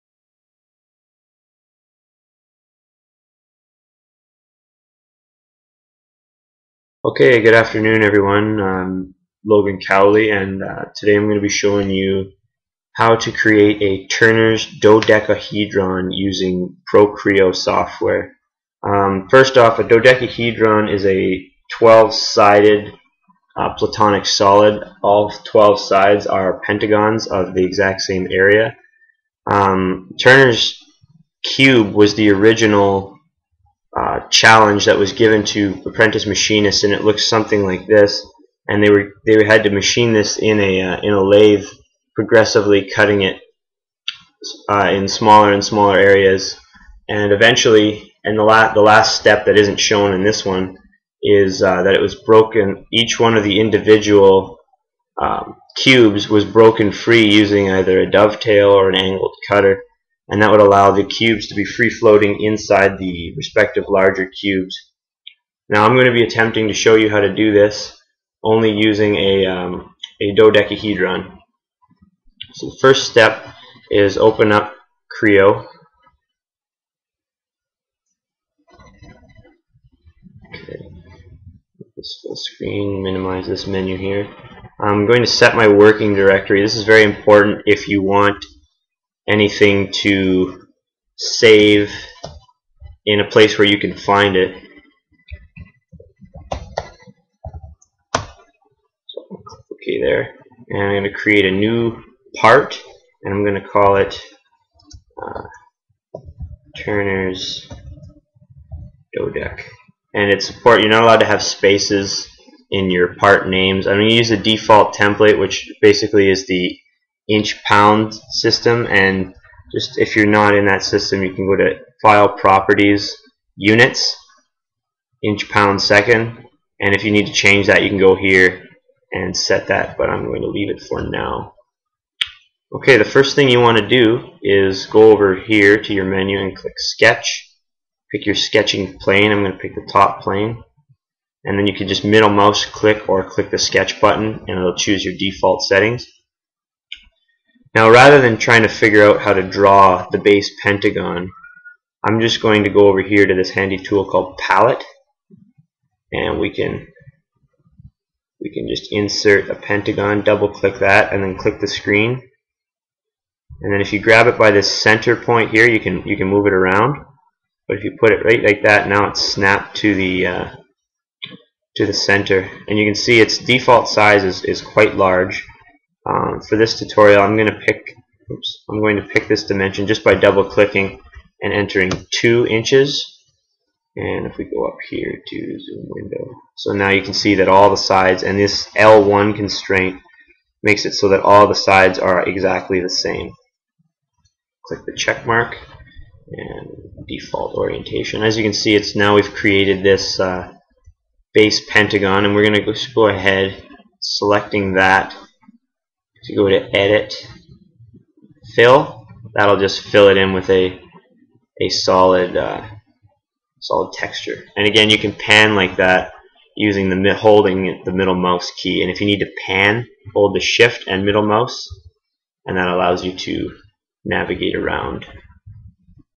Turner's Dodecahedron Part - 1
This is the first of four instructional videos on how to create the 12 sided platonic solid the dodecahedron. As well it will teach you how to use user defined functions to create the 48 holes necessary to complete the Turner's dodecahedron. I created these videos because i could not find any videos on how to make the dodecahedron or any plans for a turner's dodecahedron.